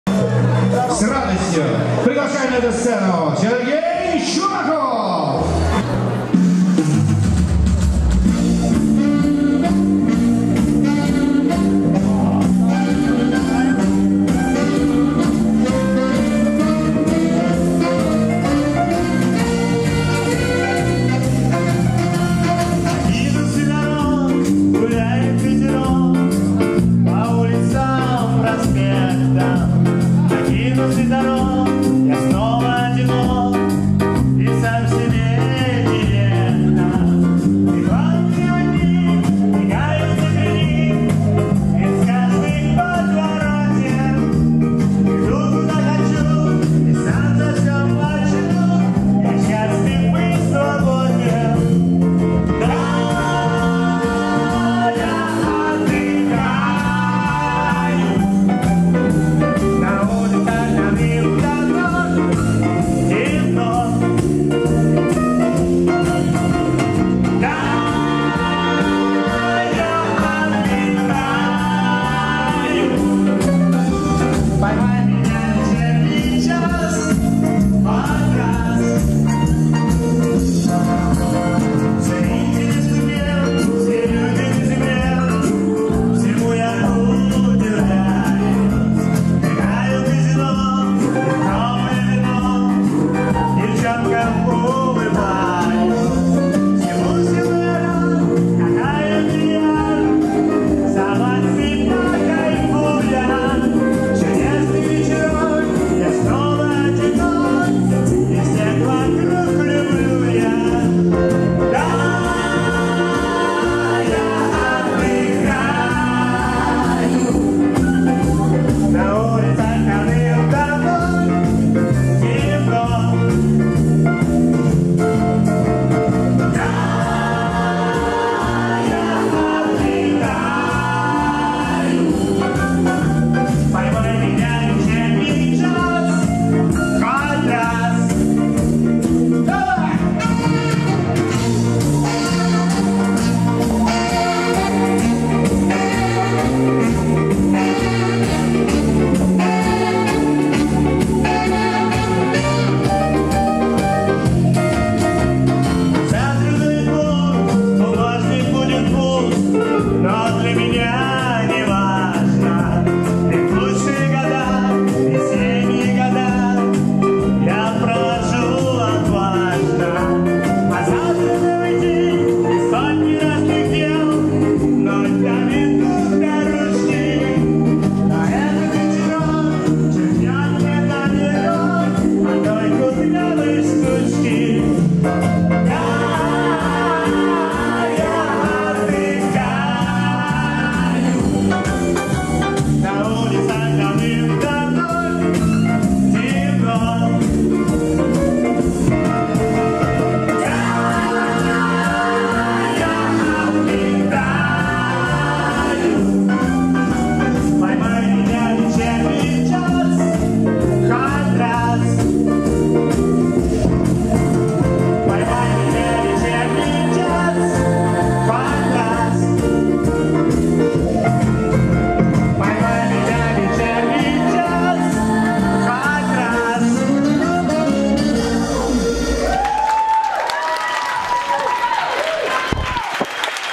Красотки на подтанцовке? Да!
24 декабря концерт в Зеленограде (клуб Сова). Билеты Встречаем Новый год вместе! ______________________________________________________________________________________________________________________________________________________________ Певец сергейчумаков принял осознанное решение приглашать на сцену красивых девушек. Им петь не обязательно. Пусть просто ходят, туда и сюда. Если хотят (могут) - пусть приседают. Или машут руками.
Во время съемок и монтажа не пострадала ни одна барышня, деревья, животные и птицы сохранены в своей природной среде.